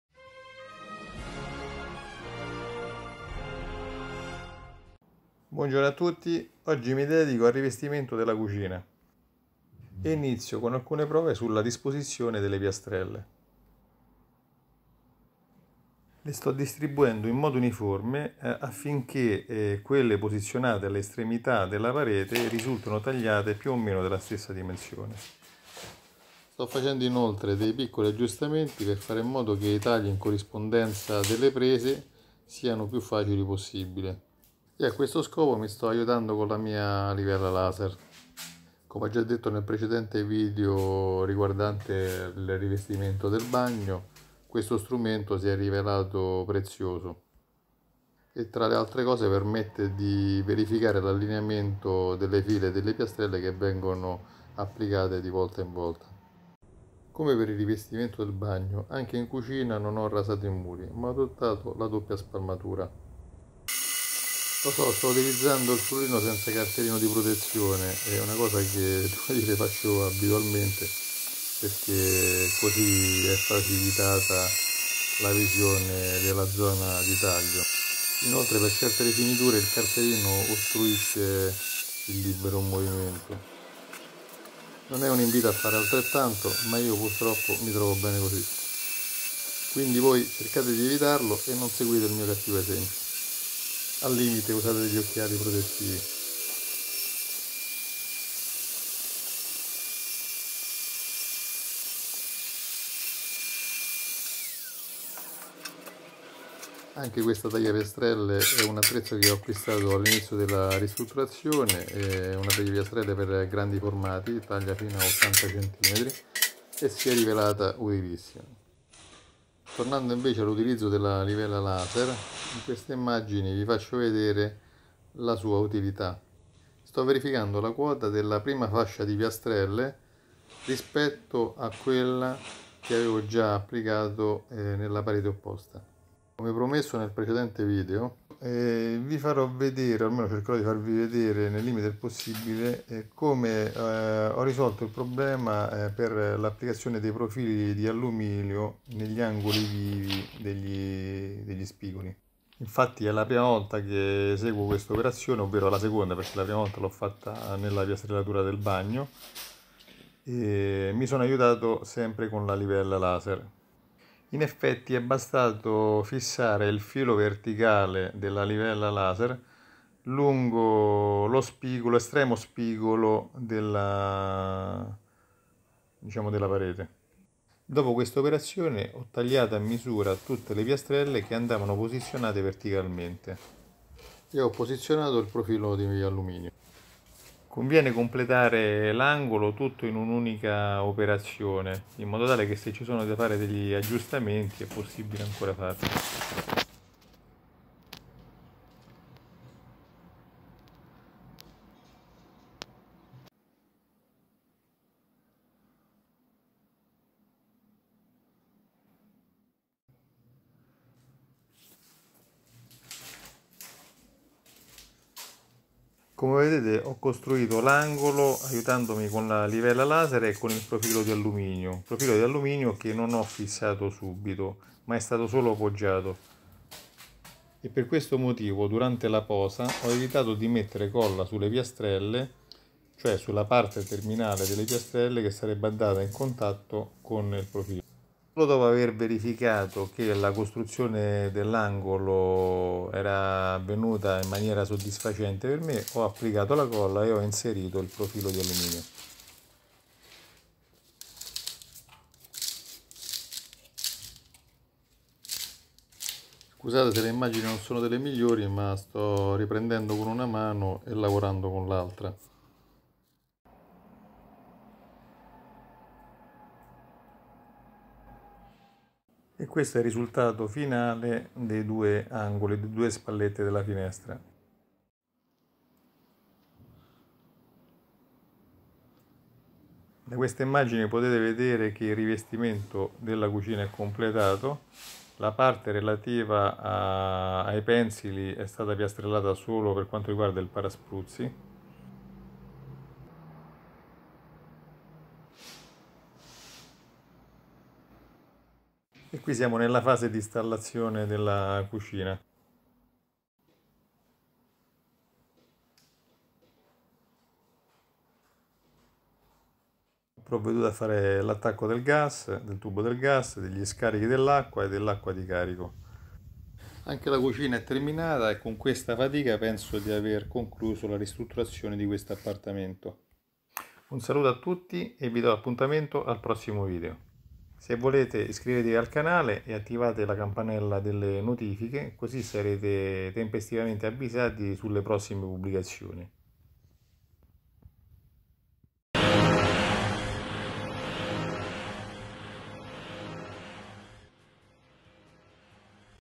Piastrellare la cucina fai da te profili angolari
Istallo il rivestimento della cucina da solo, utilizzando i livellatori per piastrelle. La fase iniziale è la più importante poiché una buona partenza influisce sul risultato finale. L’ausilio di una livella laser inoltre facilita la posa delle piastrelle. Con la livella laser ho potuto allineare più facilmente le piastrelle ed è stata fondamentale per l’istallazione  dei profili in alluminio degli spigoli vivi delle pareti. Per la posa delle piastrelle ho adottato due distinte procedure. Ho istallato la prima fila di piastrelle (quelle alla base del pavimento) tenendo allineata la base superiore con la livella laser oppure ho saltato la prima fila utilizzando un regolo e partendo direttamente dalla seconda fila. il regolo ovviamente è stato avvitato con degli stop al muro dopo aver controllato la sua orizzontalità con la livella laser ( vedi il video del rivestimento del bagno). Fra i due metodi direi che il secondo e quello che permette di realizzare un rivestimento più preciso.

I MIEI CONTATTI:
Mail:

Vai al catalogo e digita tagliapiastrelle. troverai tagliapiastrelle per grandi misure a prezzi ottimi
IL MIO CATALOGO SHOP